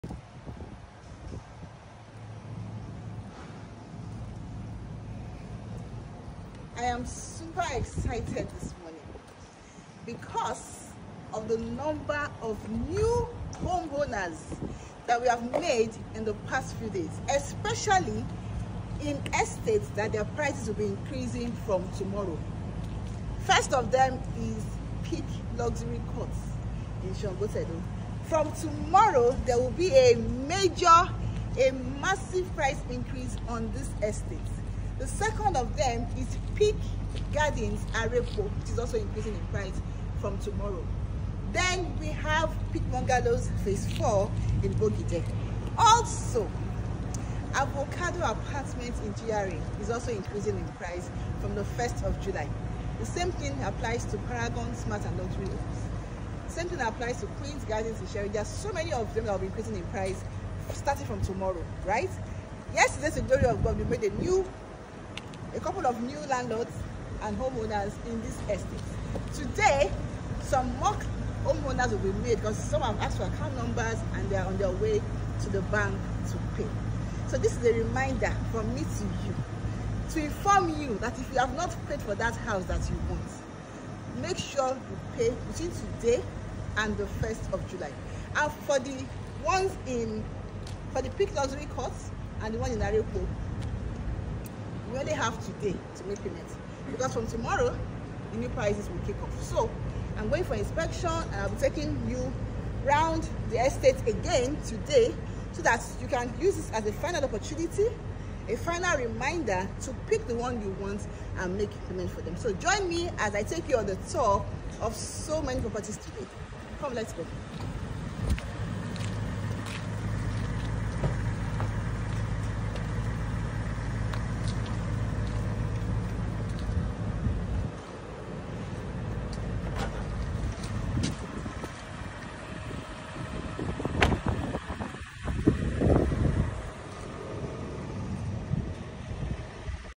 PRICE INCREASE FROM TOMORROW
PRICE IS INCREASING IN ALMOST EVERY ESTATE OOO.

Some will increase from tomorrow, and some from the 1st of July.

Try and make your deposit starting from Today.